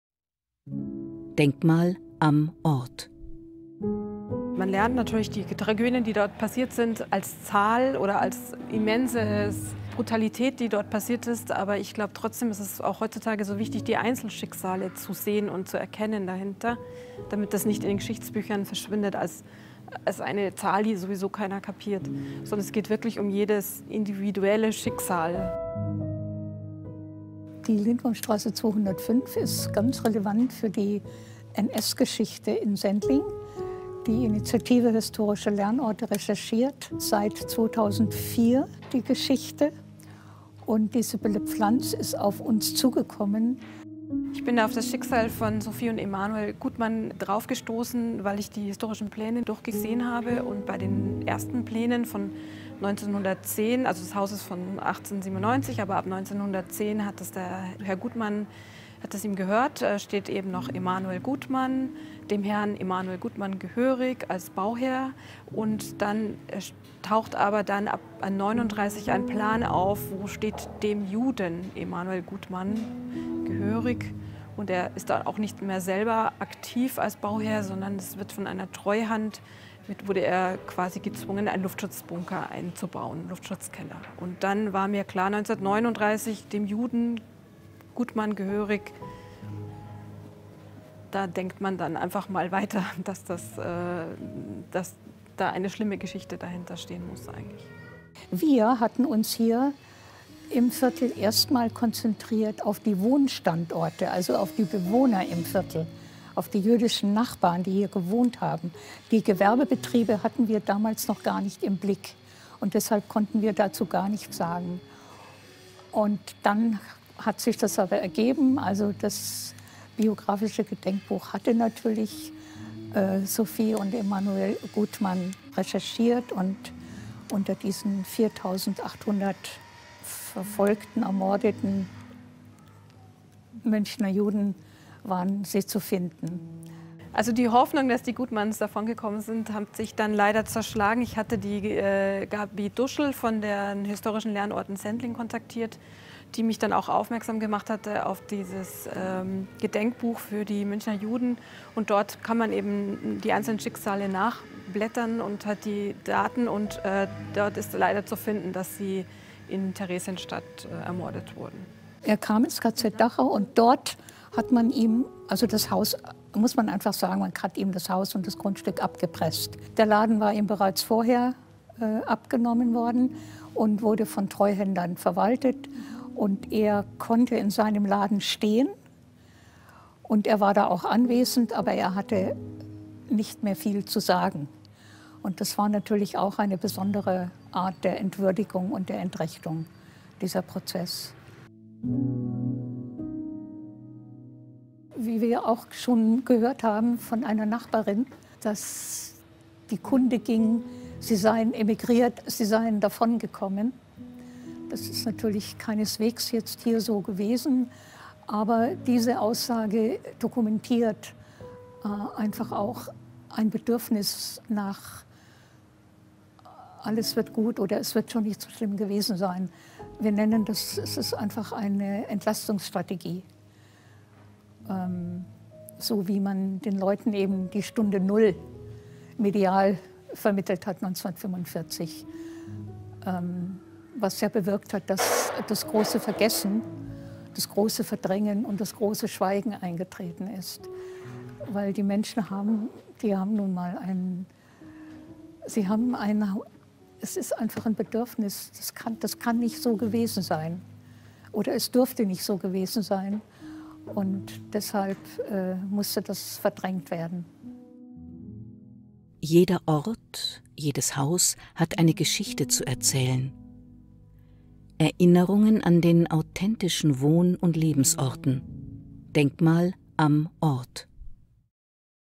Familie Gutman - DMAO München
DMAO München, 2. Mai 2021, Lindwurmstrasse 205, Textilhaus Gutman
ERINNERUNGSFILM AN DIE FAMILIE GUTMAN
von Sibylle Pflanz und Gabi Duschl-Eckertsperger

„Der Laden war vorher abgenommen worden, und wurde von Treuhänder verwaltet. Er konnte in so einem Laden stehen, war anwesend aber er hatte nicht  mehr viel zu sagen. Das war natürlich auch eine besondere Art der Entwürdigung und der Entrechtung“, so Gabi Duschl-Eckertsperger.

Emanuel und Sofie Gutman kamen nach München und eröffneten im Mai 1912 ihr Kaufhaus Gutman mit Kurz- Weiß- und Wollwaren. Das „Kaufhaus Gutman“ in Sendling wechselte zum 1. April 1934 in arischen Besitz. Mehr dazu in dem 4-minütigen Kurzfilm.

Wir danken Sibylle Pflanz und Gabi Duschl-Eckertsperger & allen an dem Film Beteiligten.
Film: Severin Vogl

Denk Mal Am Ort ist ein Projekt von KUBIN e.V.
DMAO Projektleitung München: Susana Fernández Molina

Wir danken unserem Partner Deutsche Postcode Lotterie für die Unterstützung!